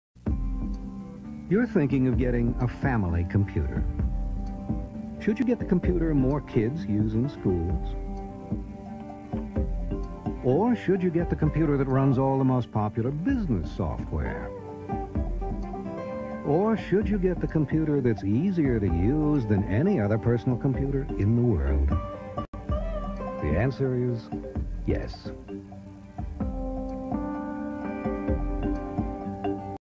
Macintosh LC 500 (1993)
The Macintosh LC 500 series is a series of personal computers that were a part of Apple Computer's Macintosh LC family of Macintosh computers. It was Apple's mid-1990s mainstream education-market Macintosh, featuring an all-in-one desktop design with a built-in 14" CRT display, CD-ROM drive, and stereo speakers. Designed as a successor to the compact Macintosh family of computers, the case is similar to the recently-introduced Macintosh Color Classic, but considerably larger and heavier due to its larger screen and a bulging midsection to house the larger electronics.